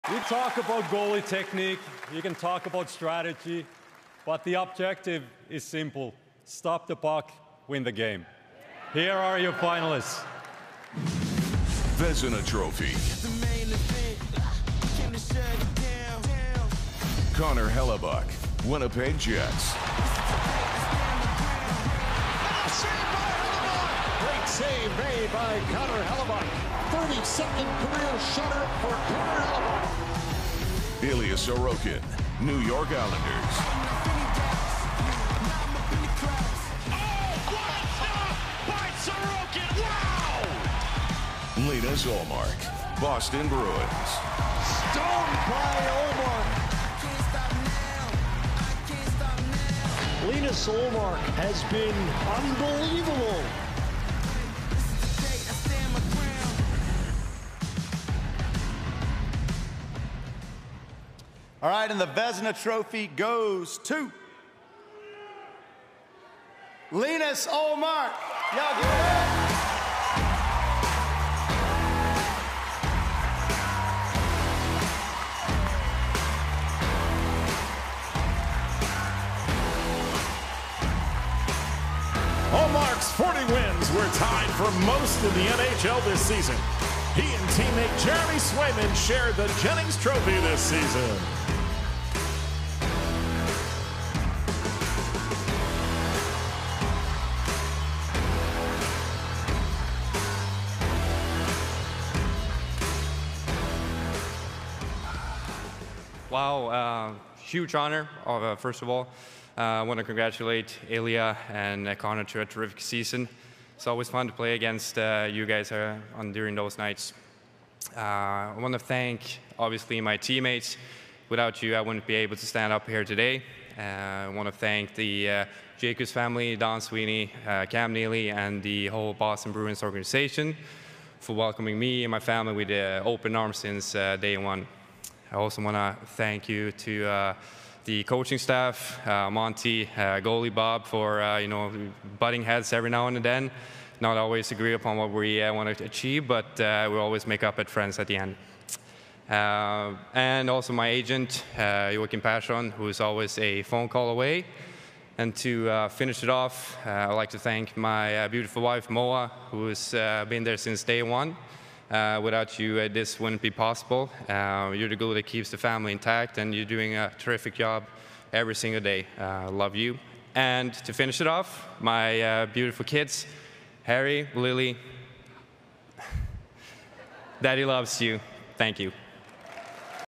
Linus Ullmark wins the 2022-23 Vezina Trophy
Bruins goalie Linus Ullmark wins the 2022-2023 Vezina Trophy